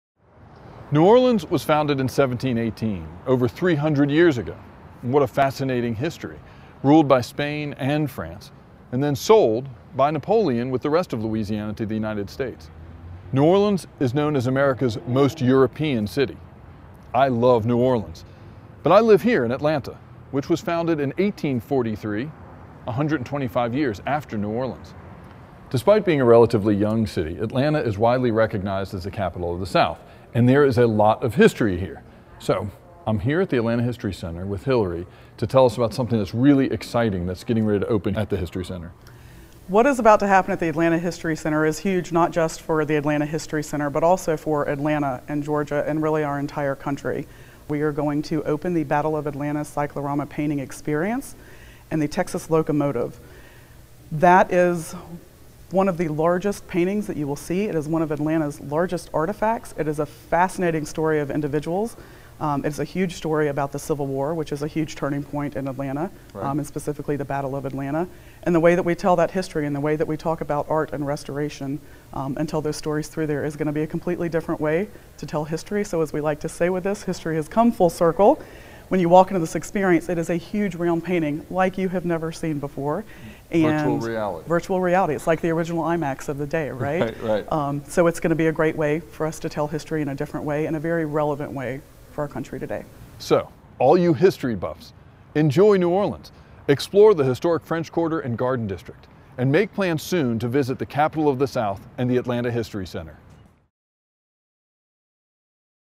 Atlanta History Center's Cyclorama Exhibit
On February 22, 2019, Atlanta History Center opens Cyclorama: The Big Picture, featuring the fully restored cyclorama painting, The Battle of Atlanta. At the centerpiece of this new multi-media experience is a 132-year-old hand-painted work of art that stands 49 feet tall, is longer than a football field, and weighs 10,000 pounds.